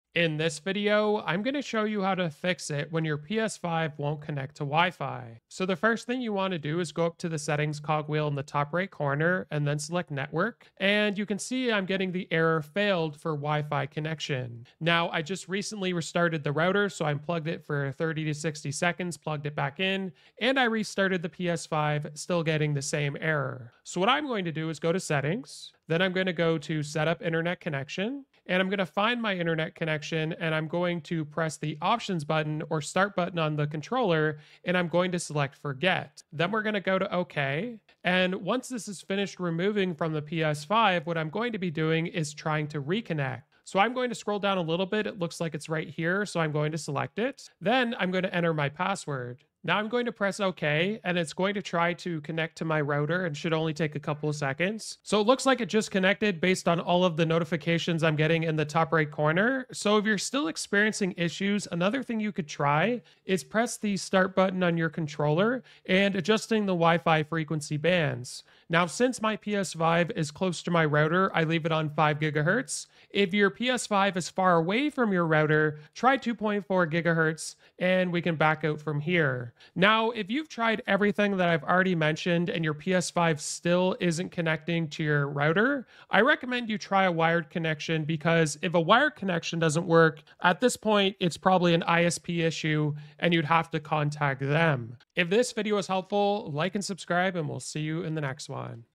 How To Fix PS5 Won't Connect To WiFi - 2024
Do you want to know how to fix it when your PS5 won't connect to your WiFi network connection and you get a failed error? In this video, I show you how to fix it when your PS5 won't connect to your WiFi internet connection.